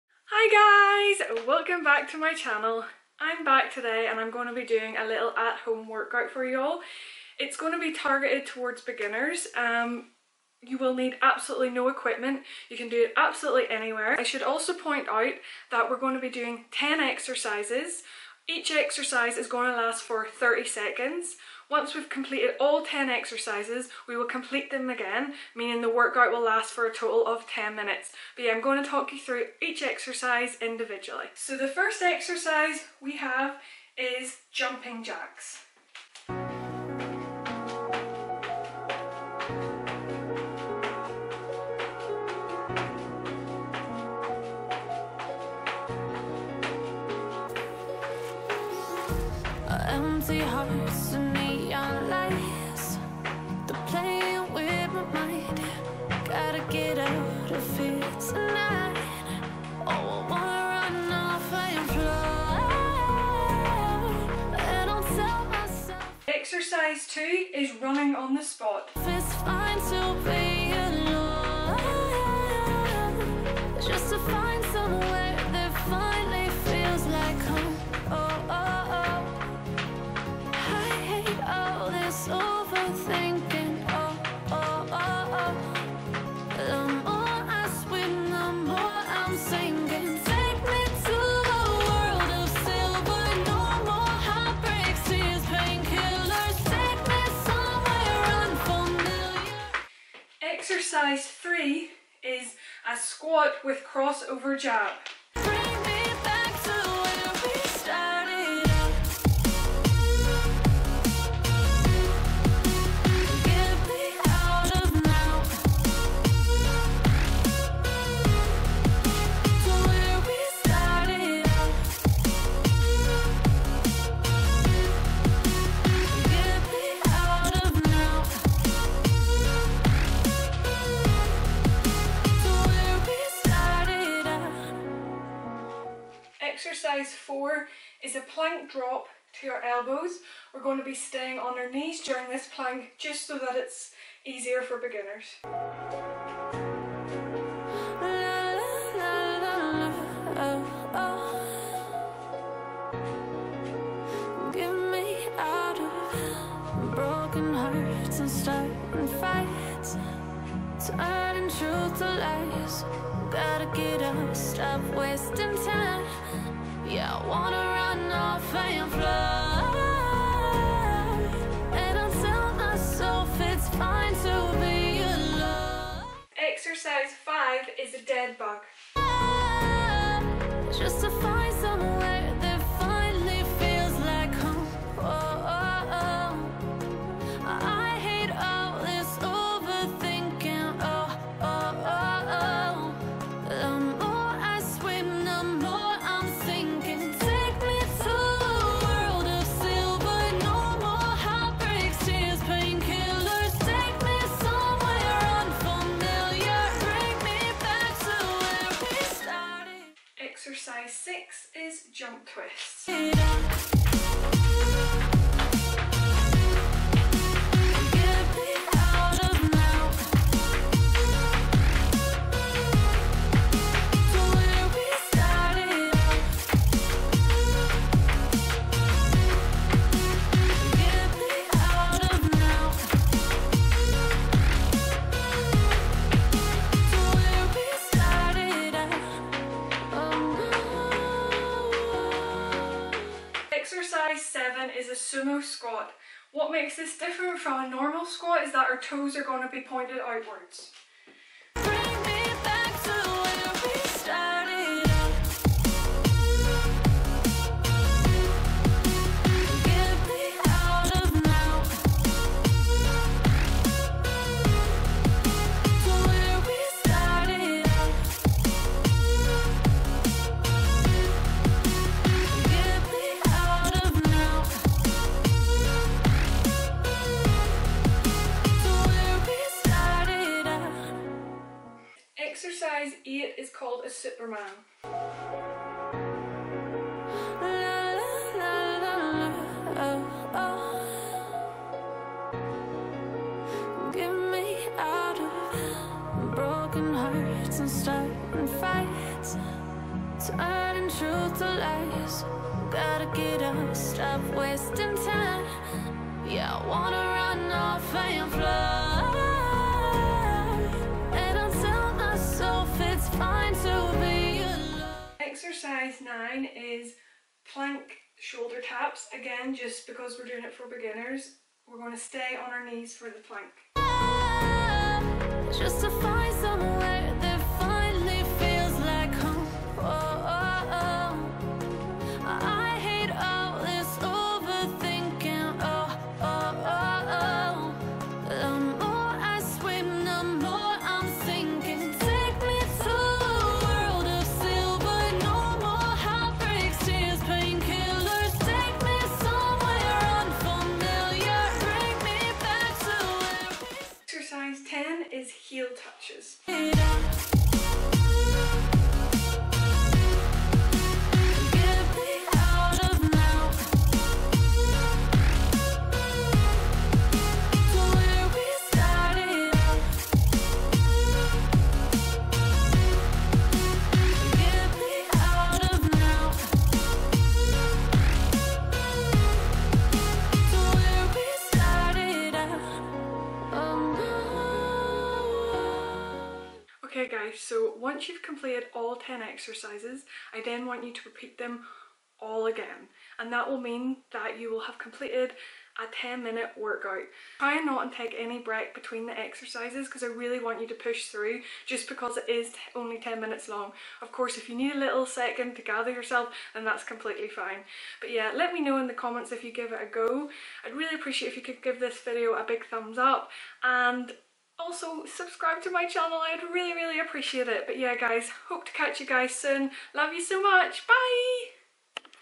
10 minute home workout for beginners (no equipment)
Hey my loves, I hope you've all had a great week. I'm back with a 10 minute at home workout. It's suitable for anyone that is just starting out and also anyone who is short of time. I also apologise for the first exercise being slightly blurry my camera didn't know where to focus.